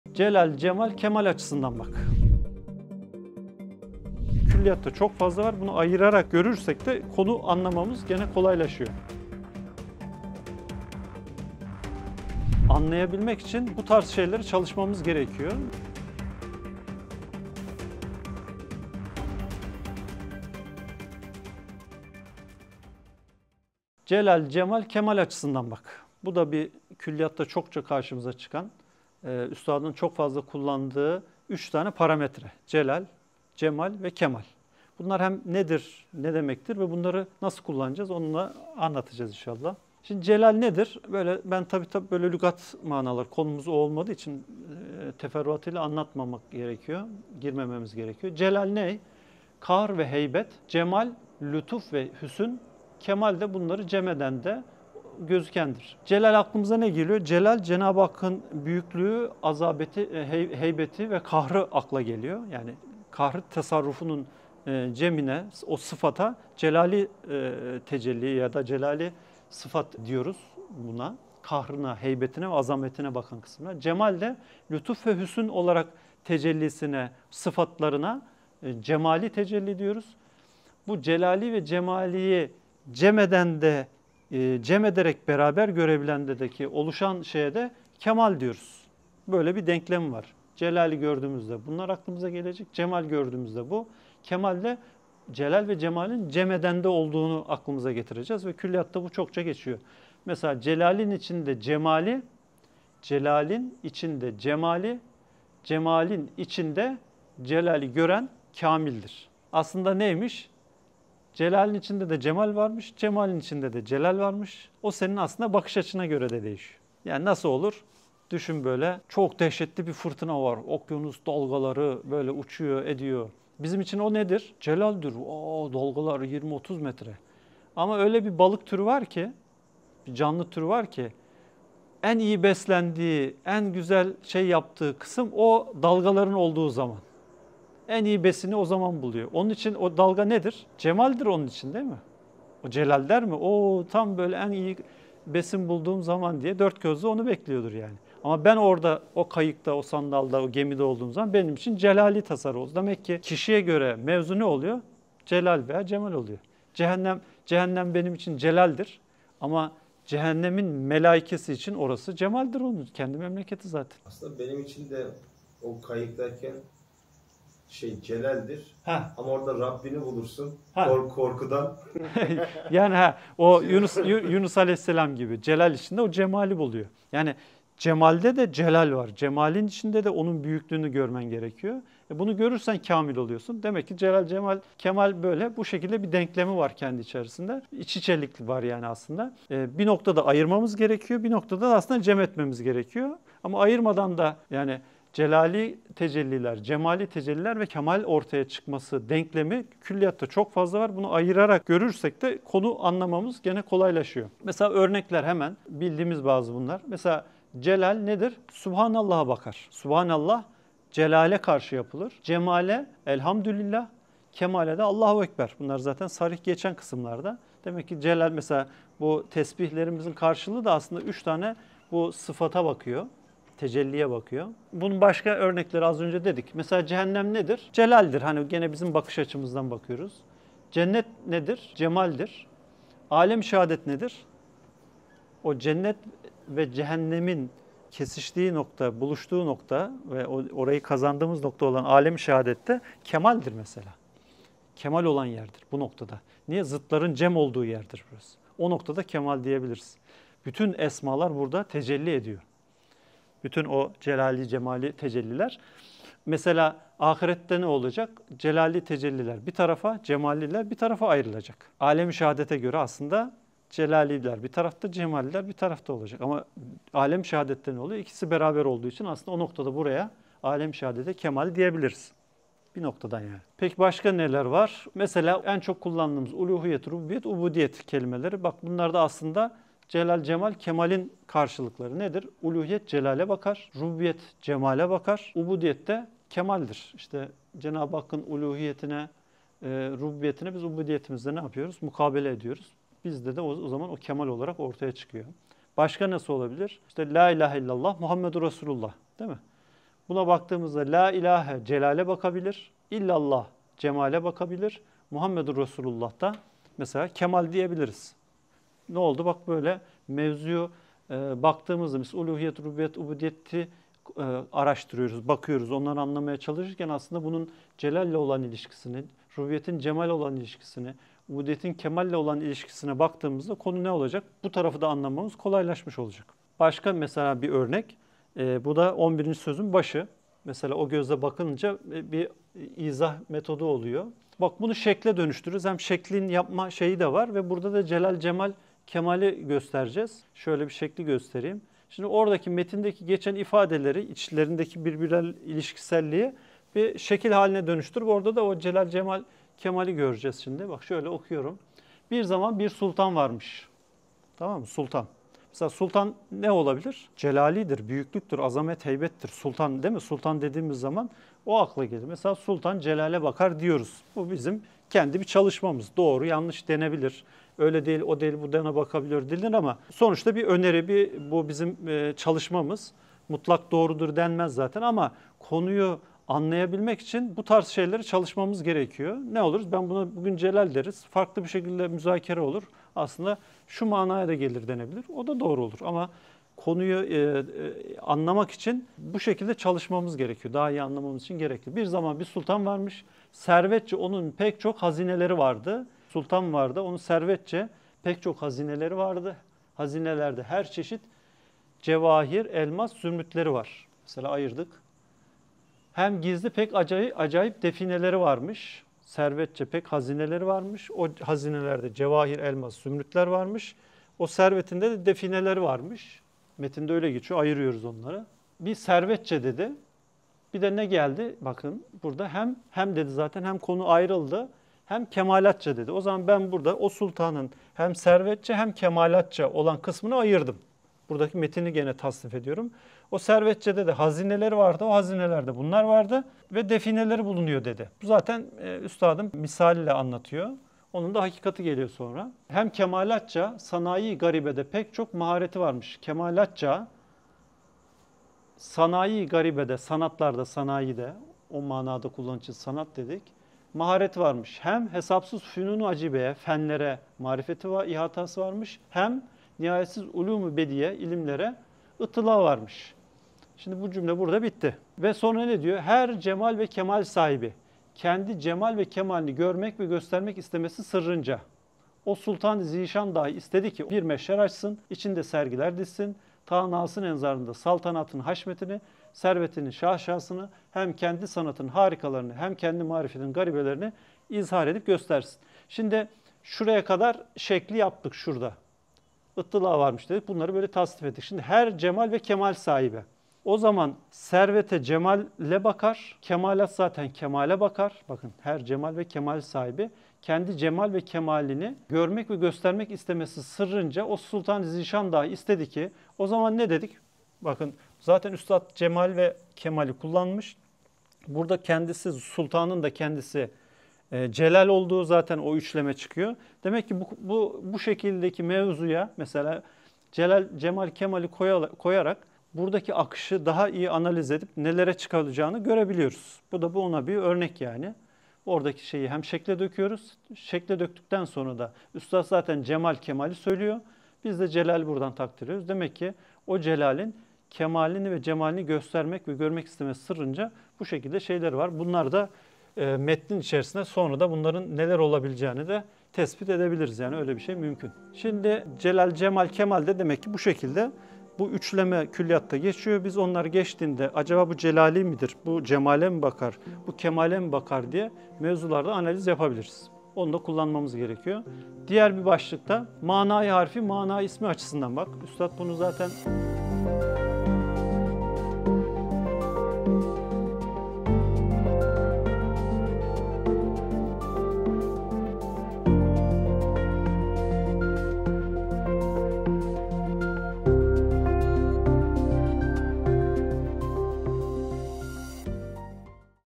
CELAL-CEMAL-KEMAL AÇISINDAN BAK | B:12
Selamun Aleyküm! Uzun zamandır beklenen ve çok sorulan bir meseleyi güzel bir çalışma hazırlamaya muvaffak olduk. RİSALE-İ NUR'DA ÇALIŞMA YÖNTEMLERİ başlığı ile başladığımız seriyi inşaAllah sizlerin istifade ve istifazalarınıza sunamaya devam ediyoruz,

Her gün saat: 17.00’da yeni bir bölümü yayınlanacak.

12.bölüm

SORULARINIZI VE ÇALIŞMALARINIZI BİZİMLE YORUMLAR KISMINDA VEYAHUT İNSTAGRAM MESAJLAR KISMINDAN ULAŞTIRABİLİRSİNİZ.